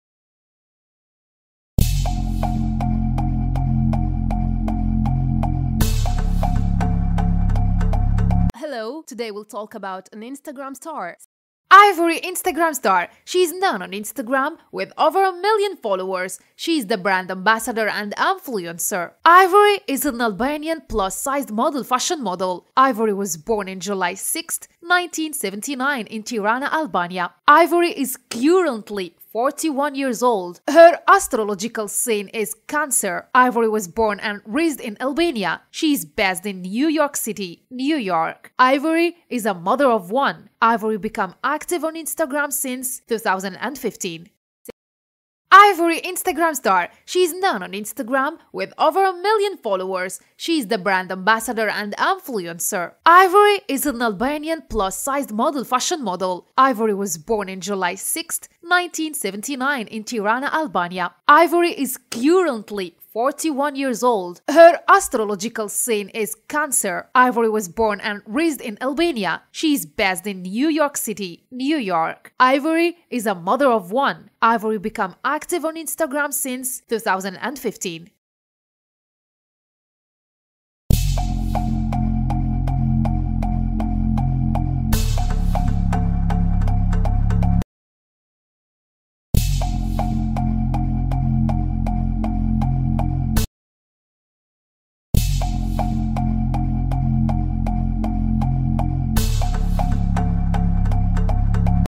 Ivory Glamorous Plus Size model Biography, lifestyle, Relationship
ivoryyyyy Instagram Model Fashion nova & Biography and Lifestyle

 IVORY sports two swimsuit looks from the Fashion Nova Curve collection.

ـــــــــــــــــــــــــــــــــــــــــــــــــــــــــــــــــــــــــــــــــــــــــ
LEGAL INFORMATION ABOUT THE CONTENT:
ــــــــــــــــــــــــــــــــــــــــــــــــــــــــــــــــــــــــــــــــــــــــــ

Add/Remove Content:  K